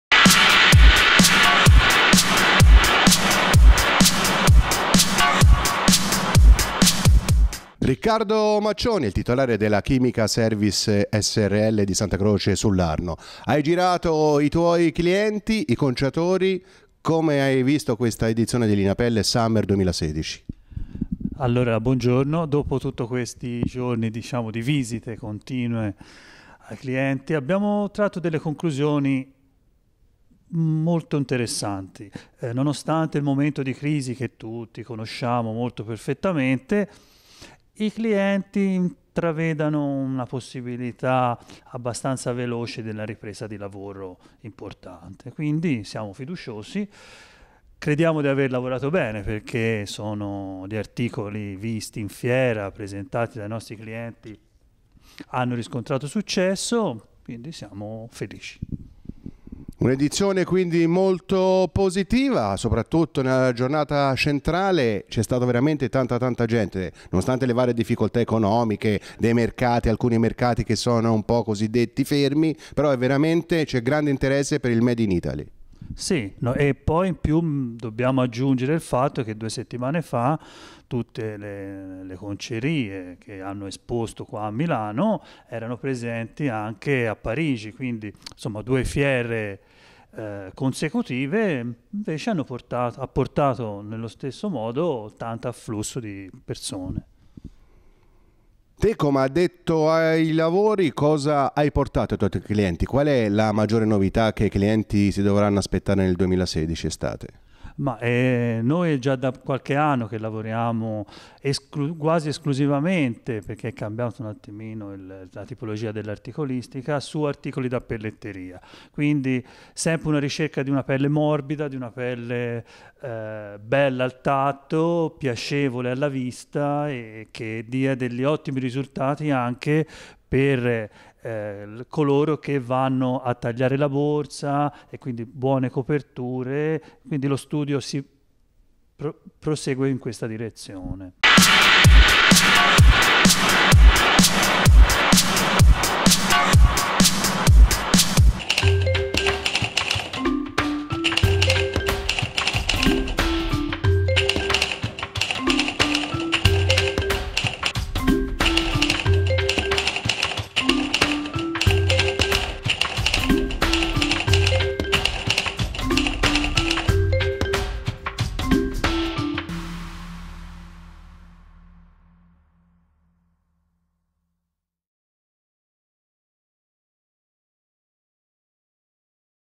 Maurizio Zini intervista a Lineapelle Milano, Riccardo Maccioni della Chimica Service
Il giornalista Maurizio Zini intervista Riccardo Maccioni della Chimica Service alla fiera di Milano - Rho, per lo speciale televisivo di Lineapelle Summer 2016 in onda sulla piattaforma satellitare SKY 879.
A Lineapelle sono mediamente presenti 1.100 aziende provenienti da 40 Paesi.
La manifestazione ha saputo diversificare la sua offerta attraverso eventi di nicchia:
Anteprima, Lineapelle New York, Lineapelle London, Tokyo, Lineapelle Asia a Canton.